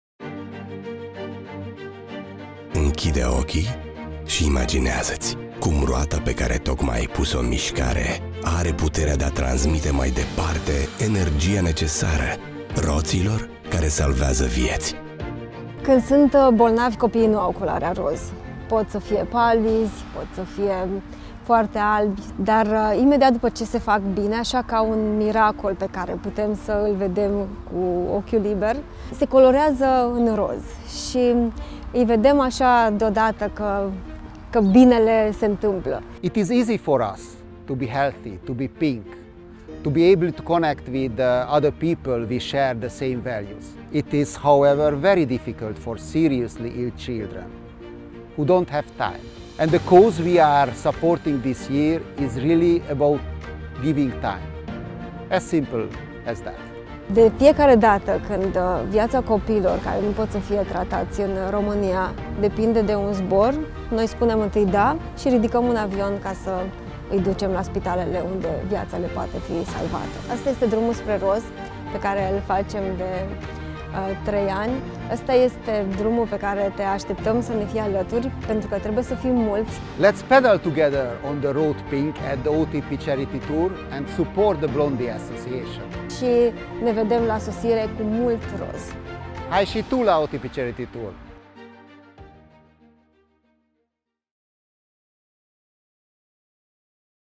OTP Charity Tour 2023
Între 19-21 mai, pedalăm împreună pe drumul spre roz la OTP Charity Tour! Dacă ești pasionat de ciclism, înscrie-te la eveniment până pe 30 aprilie și, alături de Asociația Blondie, hai să susținem zborul copiilor din România care au nevoie de operații de urgență. 🚲💚